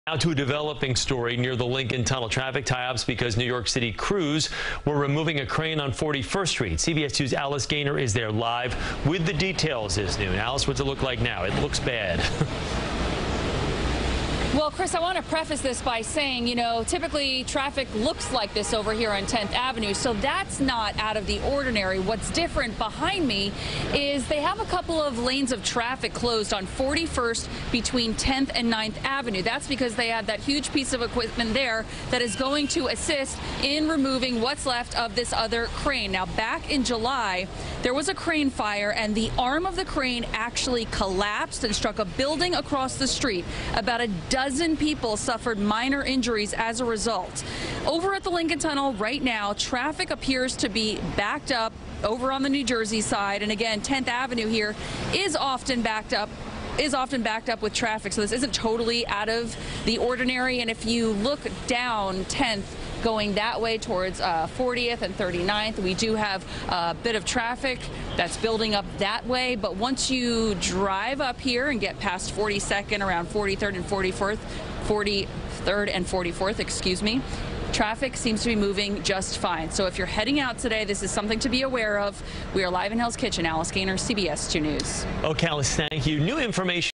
Crane removal causes major traffic delays near Lincoln Tunnel
Drivers faced heavy delays Tuesday morning at the Lincoln Tunnel as crews worked to remove the crane that caught fire back in July high above Hell's Kitchen. CBS New York's Alice Gainer has a live update from the scene.